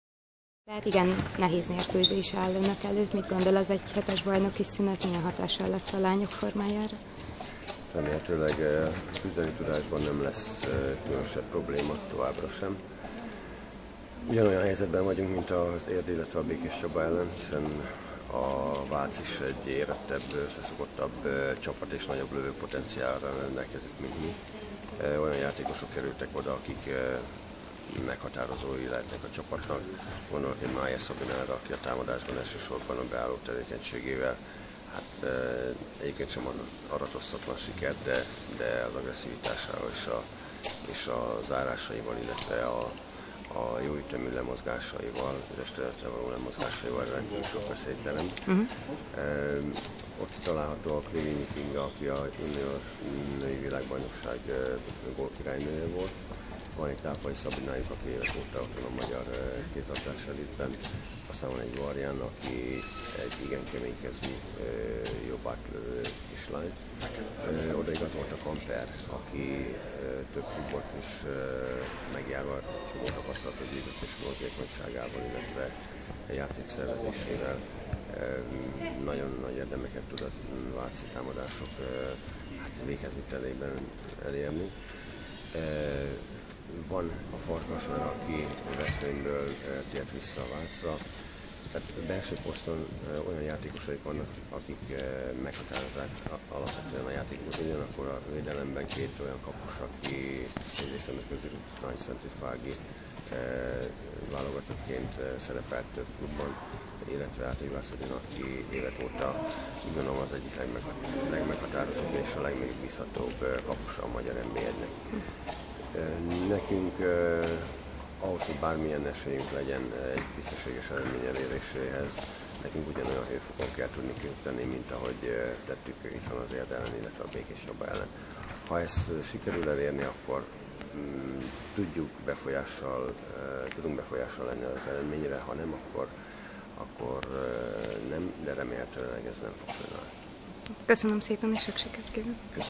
Bíró Imre
A DVSC-Fórum vezetőedzője, Bíró Imre mondja el, mit vár a pénteki Vác elleni találkozójuktól.